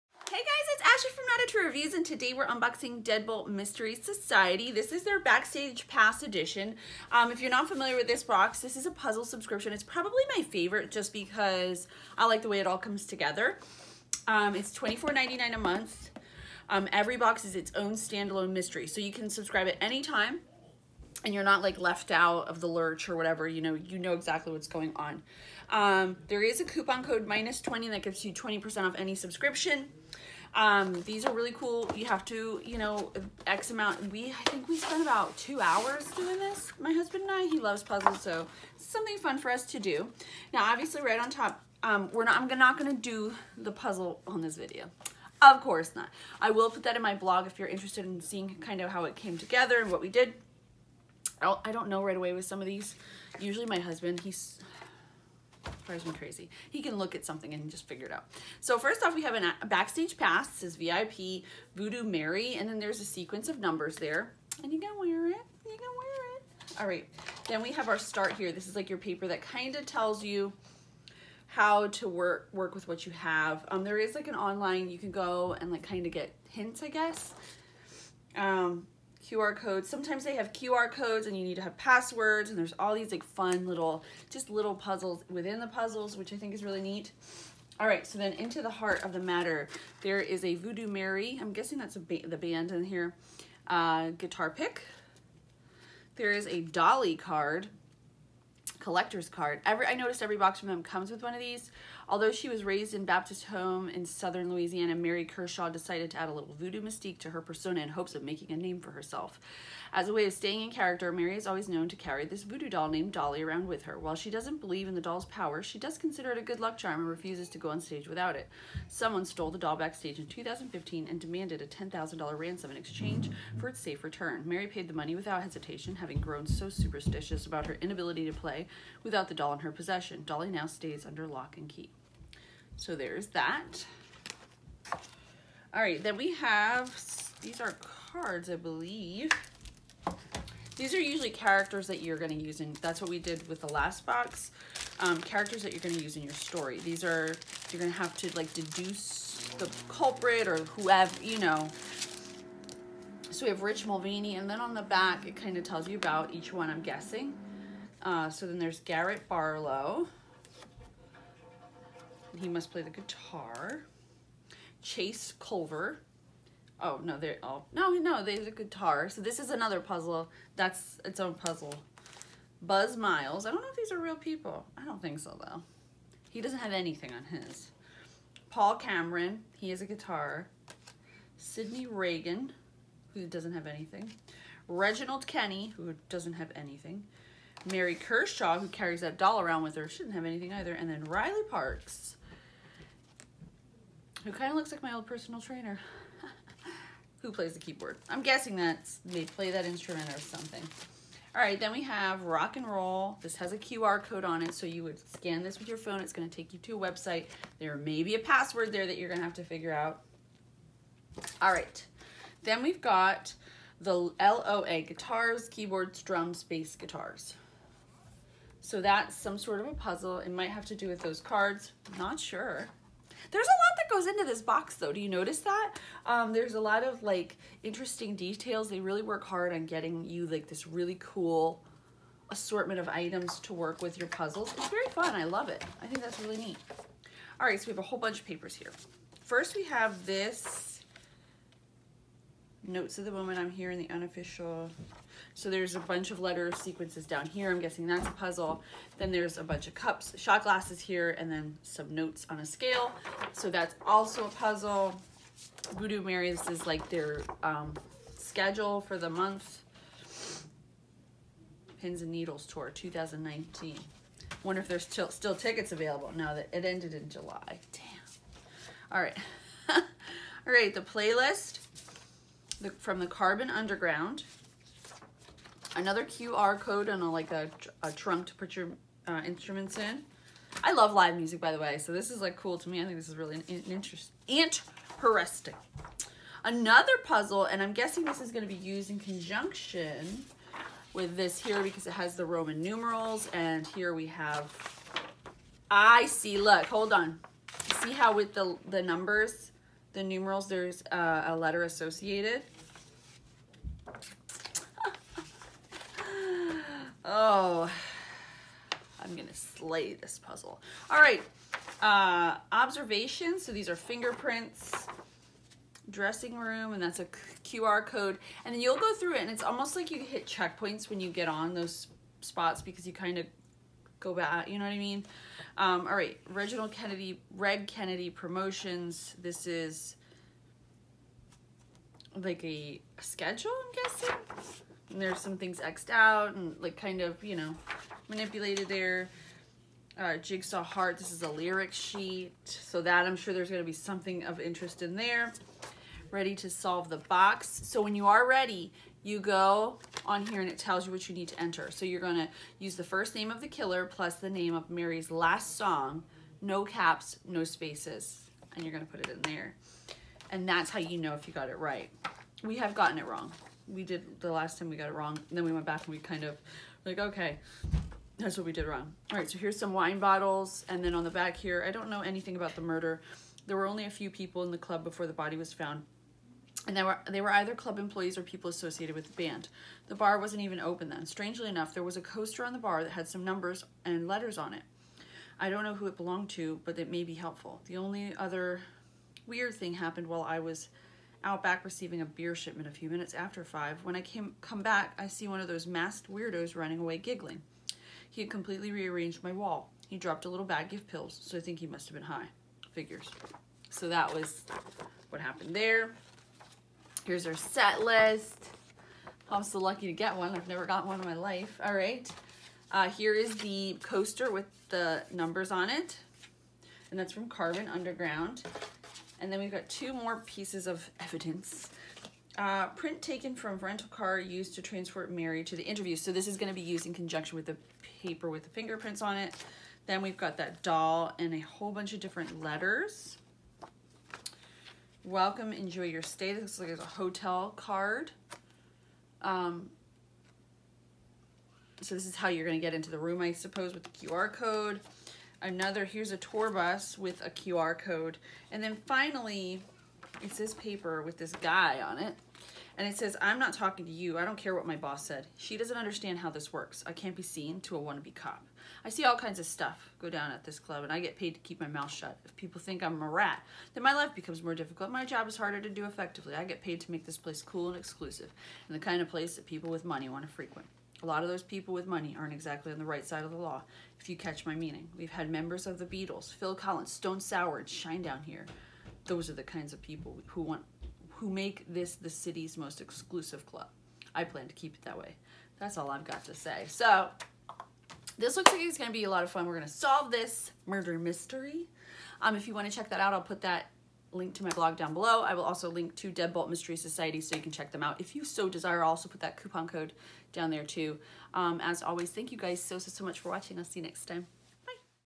Deadbolt Mystery Society "Backstage Pass" Unboxing
Deadbolt Mystery Society is a monthly puzzle subscription box. Each package contains a new stand alone mystery.